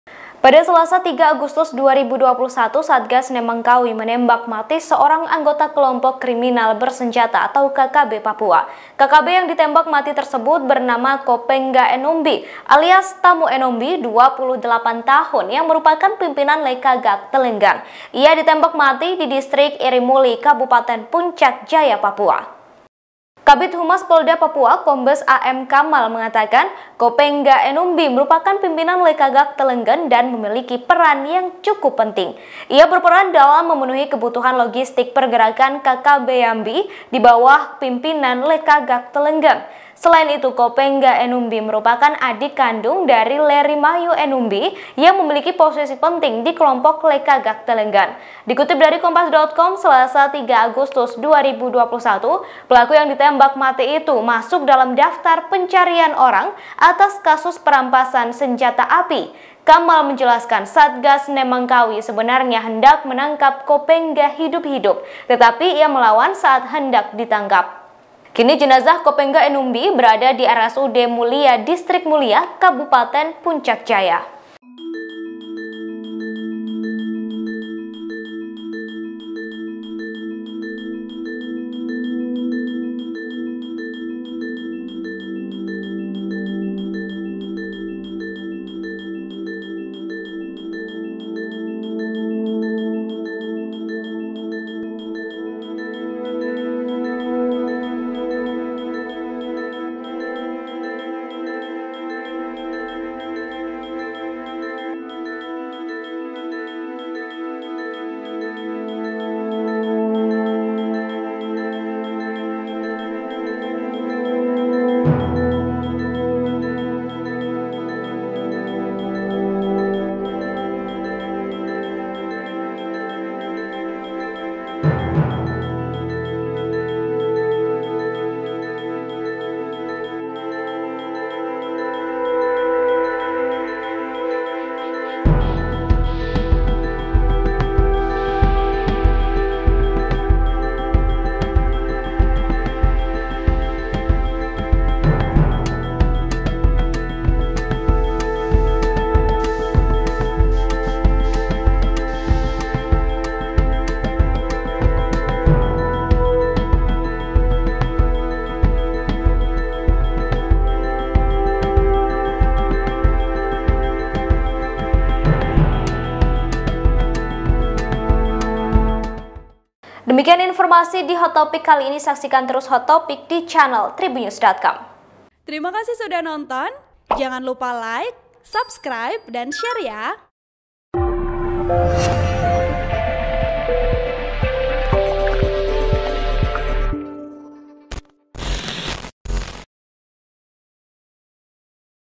Satgas Nemangkawi Tembak Mati Pimpinan KKB Papua Lekagak Telenggen, Sempat Brusaha Lawan Petugas
TRIBUN-VIDEO.COM - Pada Selasa (3/8/2021), Satgas Nemangkawi menembak mati seorang anggota Kelompok Kriminal Bersenjata (KKB) Papua.

KKB yang ditembak mati tersebut bernama Kopengga Enumbi alias Tamu Enumbi (28) yang merupakan pimpinan Lekagak Telenggen.

Ia ditembak mati di Distrik Irimuli Kabupaten Puncak Jaya, Papua.

Kabid Humas Polda Papua, Kombes AM Kamal mengatakan, Kopengga Enumbi merupakan pimpinan Lekagak Telenggen dan memiliki peran yang cukup penting.

Ia berperan dalam memenuhi kebutuhan logistik pergerakan KKB Yambi di bawah pimpinan Lekagak Telenggen.

"Selama ini, Kopengga Enumbi alias Tamu Enumbi berperan dalam memenuhi kebutuhan logistik bagi pergerakan KKB Yambi di bawah pimpinan Lekagak Telenggen," ujarnya.

Selain itu, Kopengga Enumbi merupakan adik kandung dari Lerimayu Enumbi yang memiliki posisi penting di kelompok Lekagak Telenggen.

Dikutip dari Kompas.com, Selasa (3/8/2021), pelaku yang ditembak mati itu masuk dalam daftar pencarian orang atas kasus perampasan senjata api.

Kamal menjelaskan, Satgas Nemangkawi sebenarnya hendak menangkap Kopengga hidup-hidup, tetapi ia melawan saat hendak ditangkap.

"Saat dilakukannya penindakan, terdapat perlawan dari pelaku sehingga personel mengambil tindakan tegas terukur dengan menembak Kopengga Enumbi alias Tamu Enumbi yang mengakibatkan pelaku meninggal dunia di tempat," kata dia.

Kini, jenazah Kopengga Enumbi berada di RSUD Mulia, Distrik Mulia, Kabupaten Puncak Jaya.

(Tribun-Video.com/Kompas.com)

Artikel ini telah tayang di Kompas.com dengan judul "Anggota KKB yang Punya Peran Penting Ditembak Mati, Sempat Melawan Saat Hendak Ditangkap", Klik untuk baca
Penulis : Kontributor Jayapura, Dhias Suwandi
Editor : Robertus Belarminus